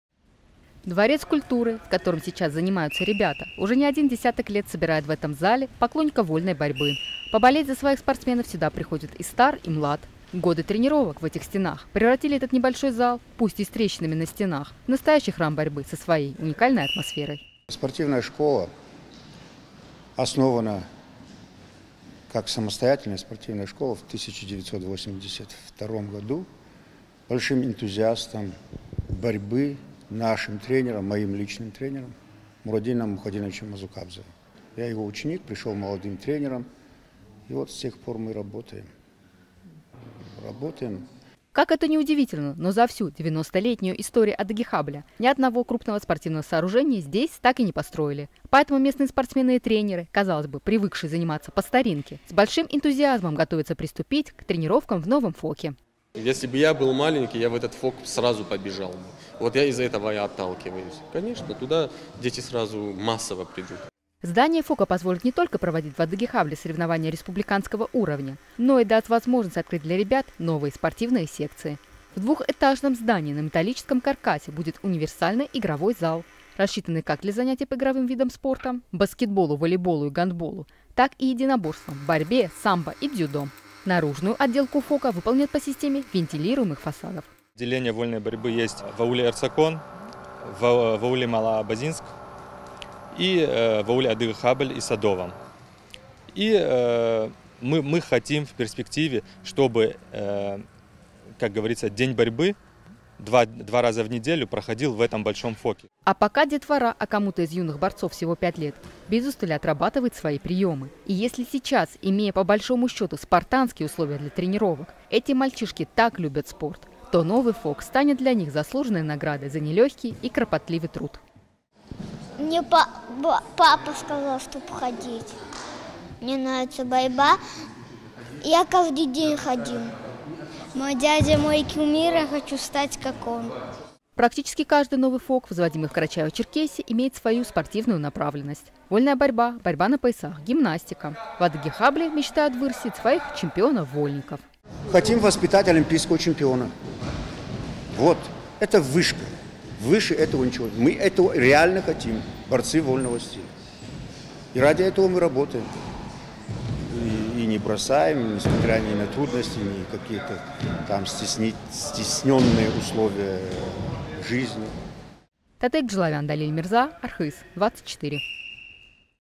В ауле Адыге-Хабль началось строительство многофункционального ФОКа
Первый в ауле и даже в районе! Новый современный многофункциональный физкультурно-оздоровительный комплекс с универсальным игровым залом вскоре появится у жителей аула Адыге-Хабль. Как будет оснащен новый ФОК? Кто будет в нем заниматься? Какую роль играют новые спортивные объекты в развитии республики? Обо всем этом расскажет наш корреспондент.